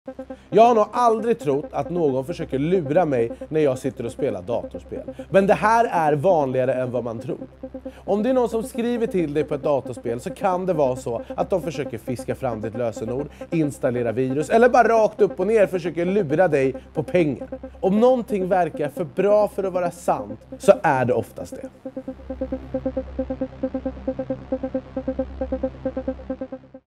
Anis Don Demina: Lurad i datorspel - vanligare än man tror
“Jag har nog aldrig trott att någon försöker lura mig när jag sitter och spelar datorspel. Men det är vanligare än man tror.”

Artisten Anis Don Demina berättar varför det är viktigt att vara försiktig på nätet.

Tänk Säkert: En kampanj på uppdrag av regeringen för ett säkrare digitalt samhälle.

Partners:
Myndigheten för samhällsskydd och beredskap. Polisen. Internetstiftelsen.